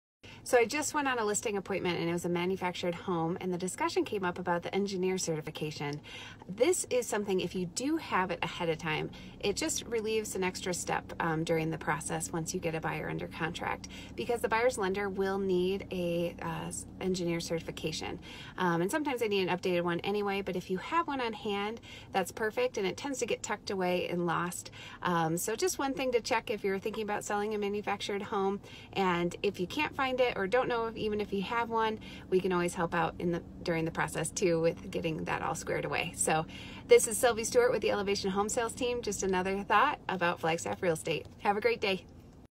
When selling a manufactured home, what should you consider?
Do you have your engineer certification for your manufactured home?

We can help out getting that all squared away! We are here to work for you!